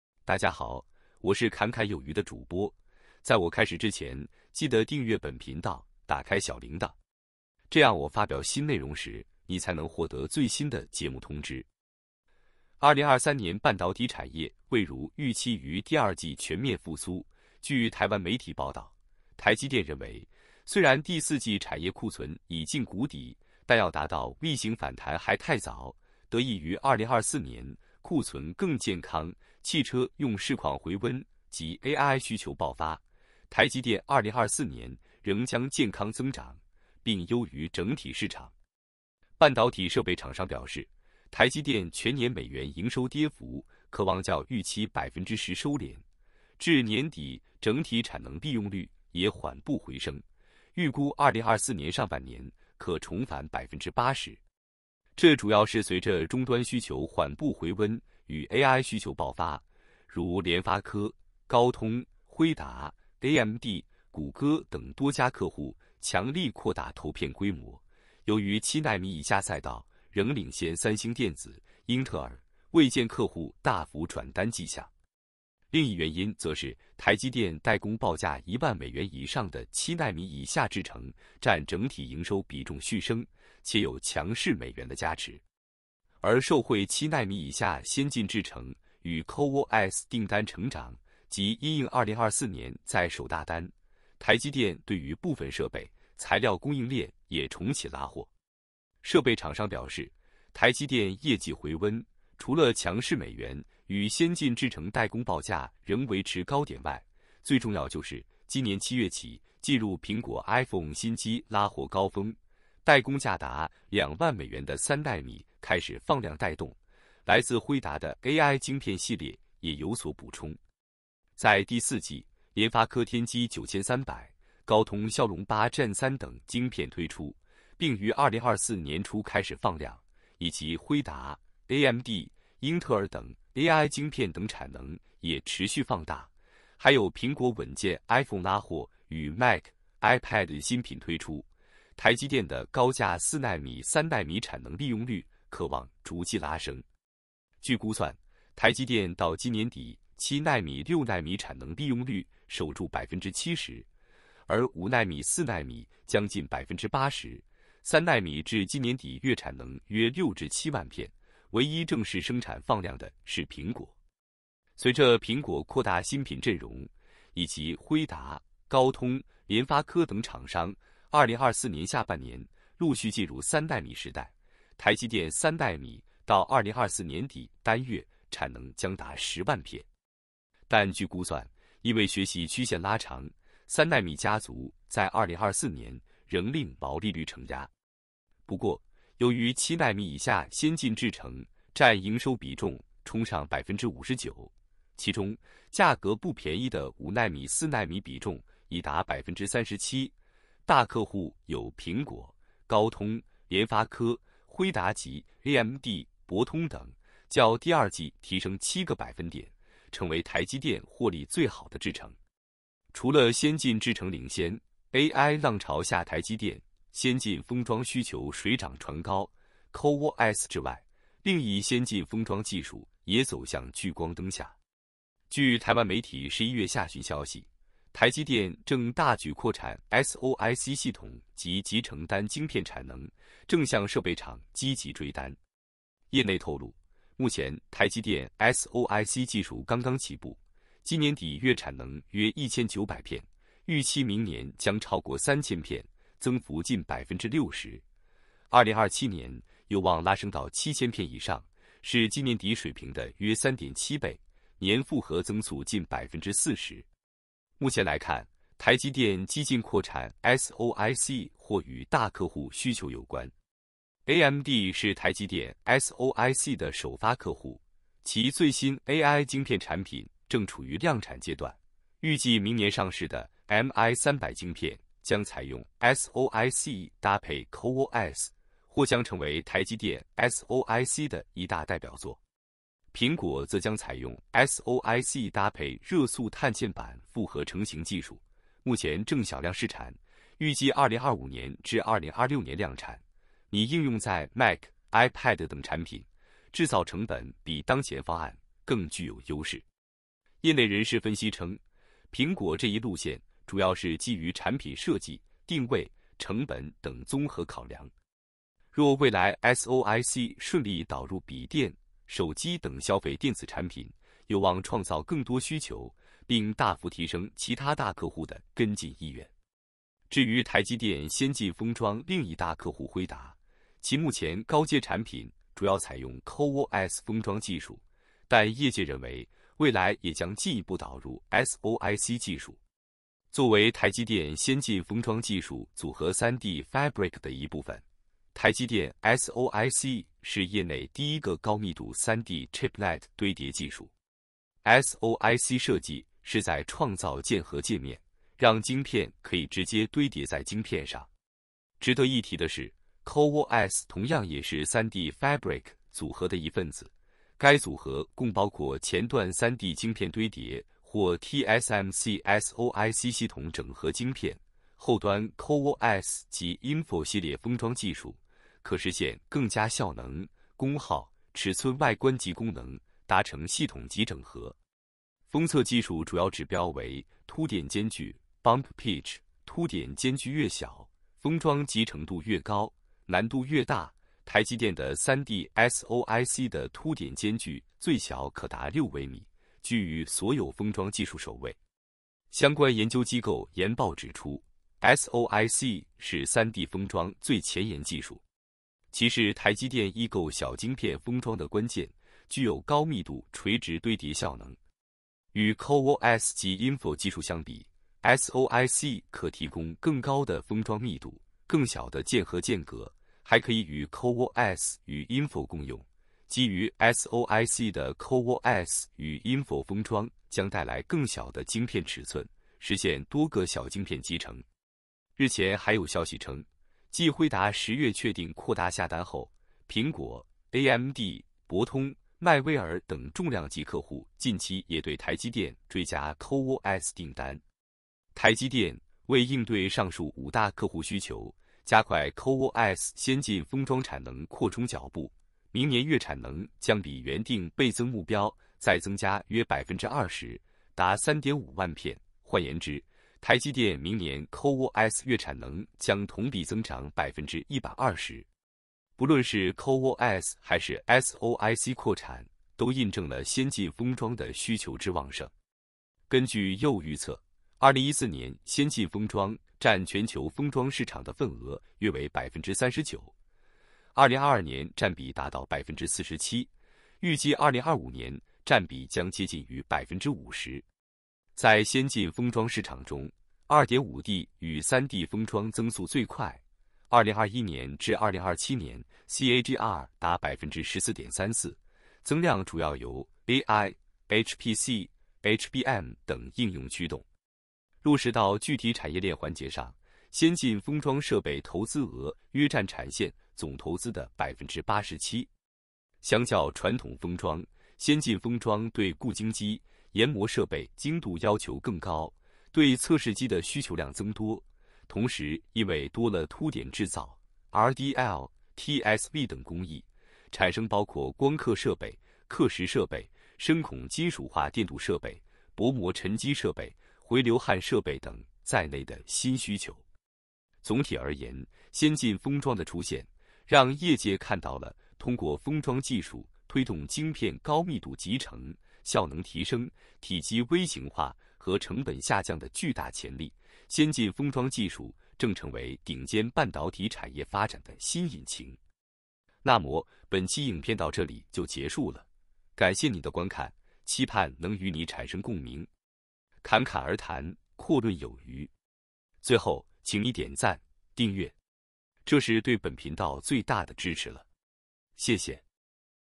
押爆賺麻！台積電超級加碼3D晶片封裝最前沿，SoIC產能巨增60%。AI浪潮下不止CoWoS激增，3D Fabric帶動高密度3D Chiplet堆疊技術。AMD MI300系列首發徹底爽哭。
半導體設備廠商表示，台積電全年美元營收跌幅可望較預期10%收斂，至年底整體產能利用率也緩步回升，預估2024年上半年可重返80%。隨著蘋果擴大新品陣容，以及輝達、高通、聯發科等廠商2024年下半年陸續進入3nm時代，台積電3nm到2024年底單月產能將達10萬片。AI浪潮下台積電先進封裝需求水漲船高，CoWoS之外，另一先進封裝技術也走向聚光燈下。據台灣媒體11月下旬消息，台積電正大舉擴產SoIC系統級集成單晶片產能，正向設備廠積極追單。